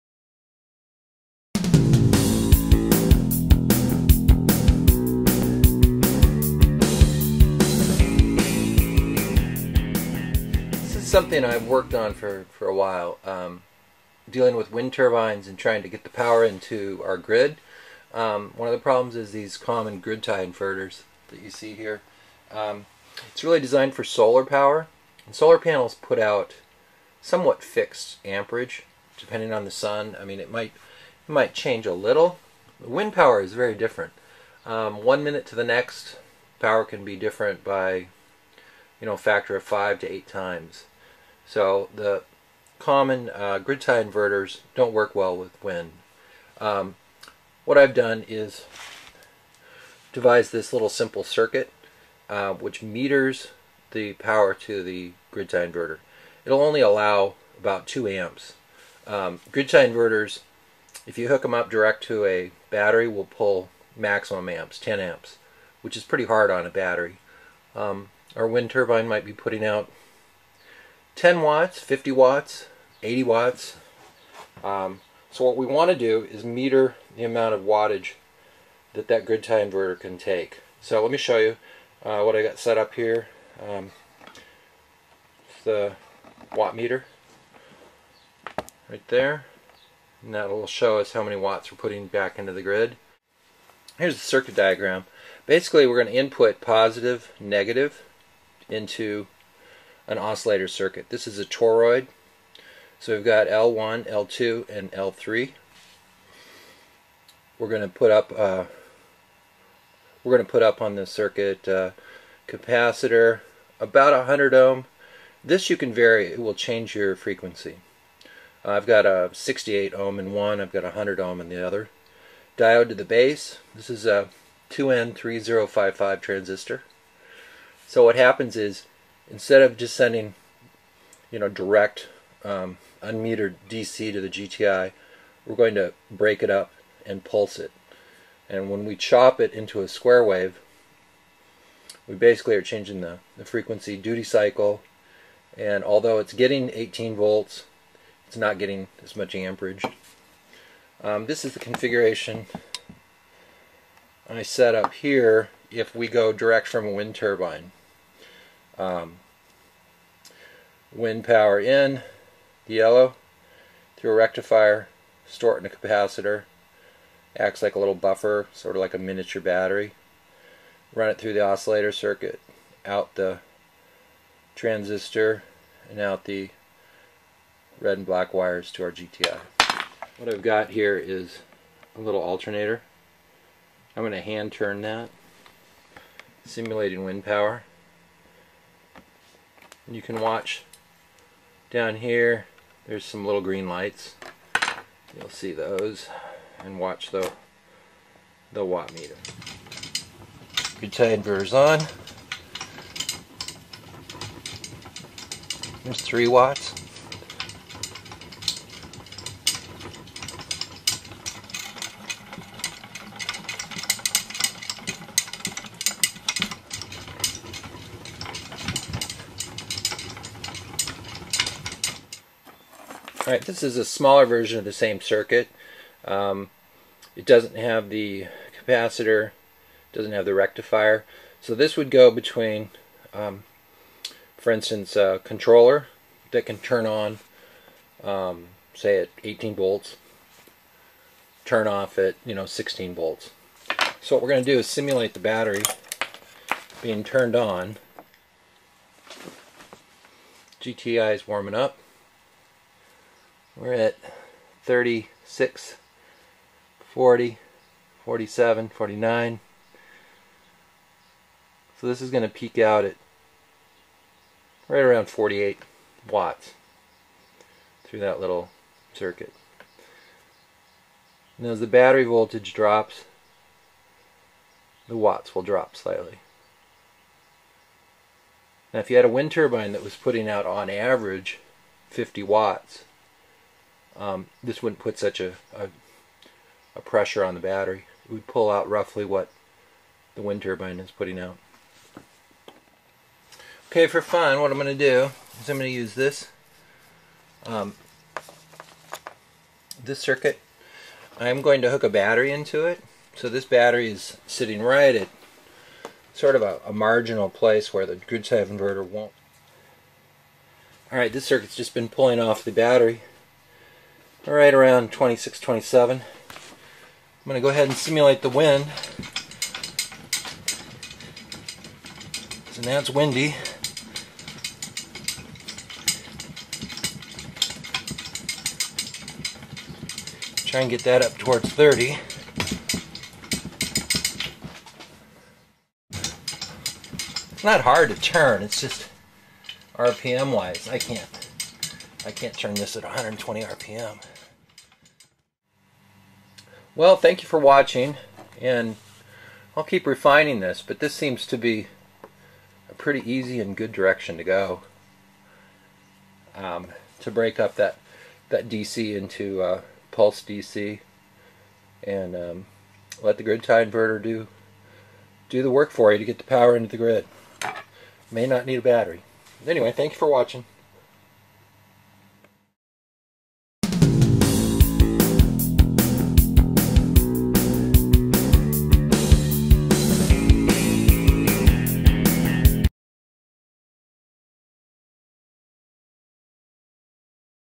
Lynx Wind - Grid Tie Inverter Limiter
This circuit is experimental but seems to be a good direction for limiting the amount of amps a standard solar GTI can pull from a battery being charged by a wind turbine.  It also seems to do a good job without the battery at all.